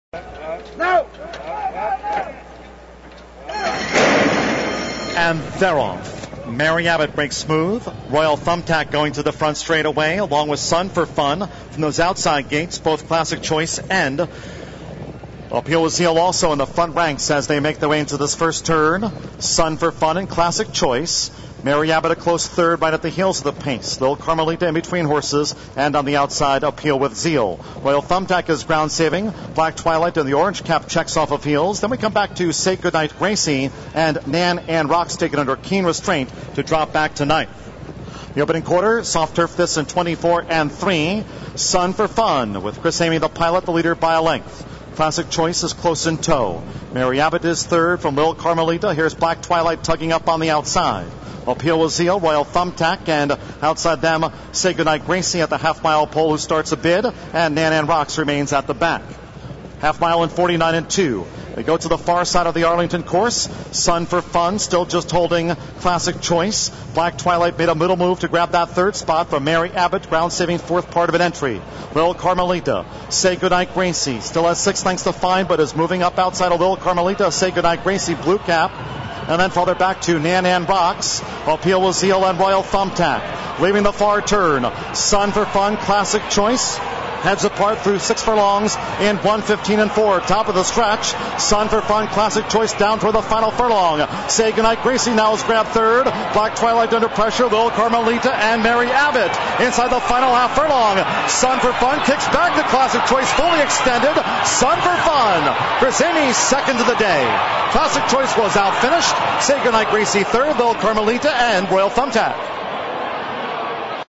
ARLINGTON PARK, 2008-09-11, Race 5
Entries: Thoroughbred Race, ARLINGTON PARK, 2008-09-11, Race 5:
      (Pgm-Runner-Jockey-Finish)
       1-Saygoodnightgracie (IL), Lopez, U, finished 3;
       1A-Mary Abbott (IL), Perez, E, finished 8;
       2-Royal Thumb Tack (IL), Meier, B, finished 5;
       3-Nan Ann Rocks (IL), Razo, Jr., E, finished 9;
       4-Lil Carmelita (IL), Alvarado, J, finished 4;
       5-Sun for Fun (IL), Emigh, C, finished 1;
       6-Black Twilight (IL), Graham, J, finished 6;
       7-Appeal With Zeal (IL), Sterling, Jr., L, finished 7;
       8-Classic Choice (IL), Karlsson, I, finished 2;